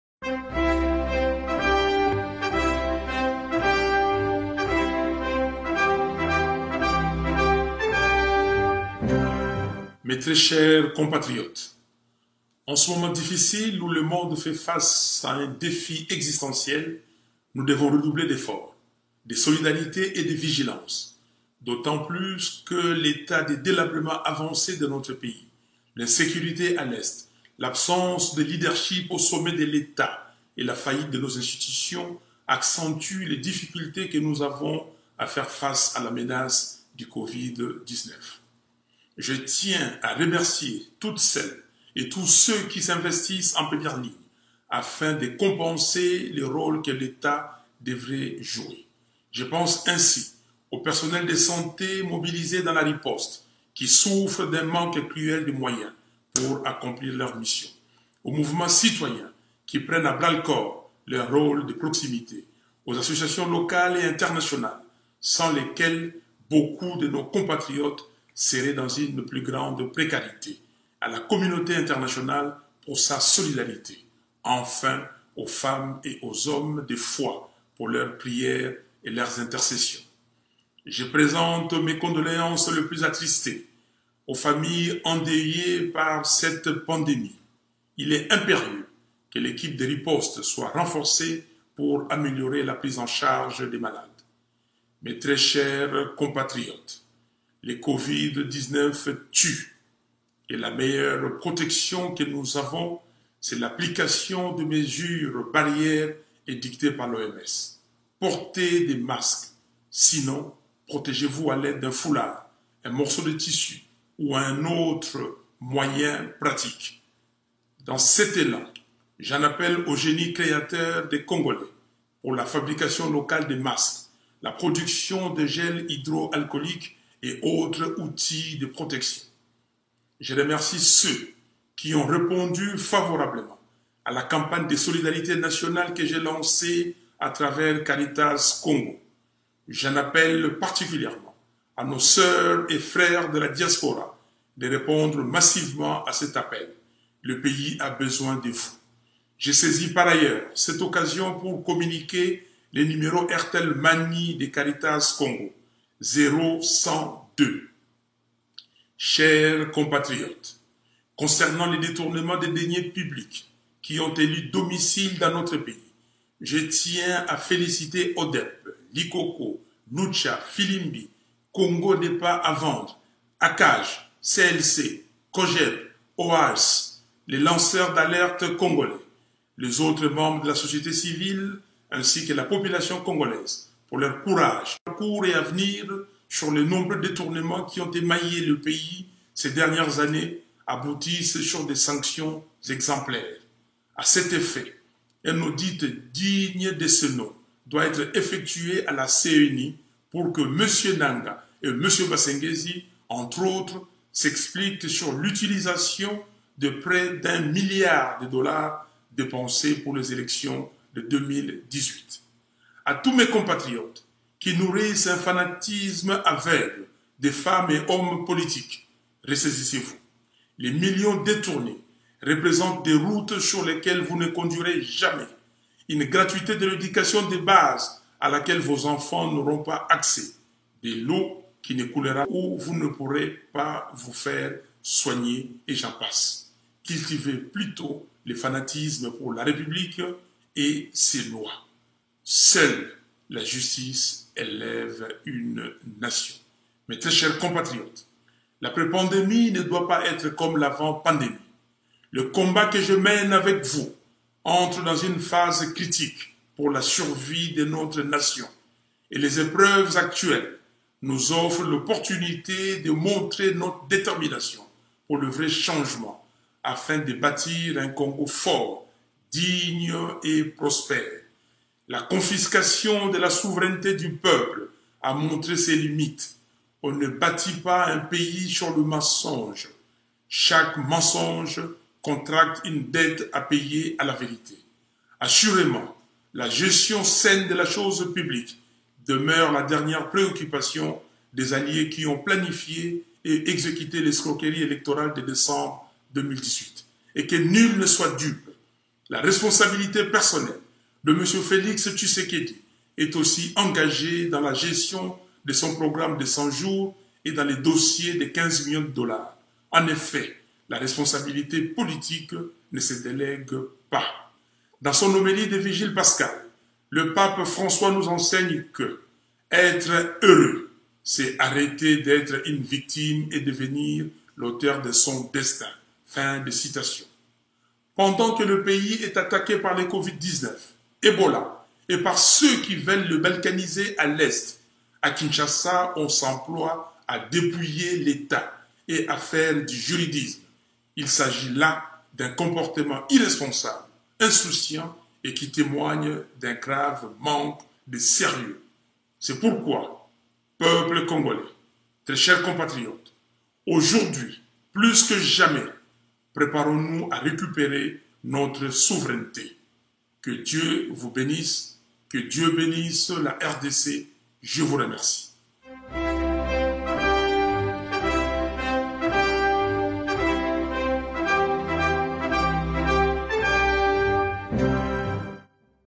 MARTIN FAYULU APPEL LES CONGOLAIS A LA VIGILANCE ET A LA SOLIDARITÉ
Adresse a la nation congolaise par Martin FAYULU madidi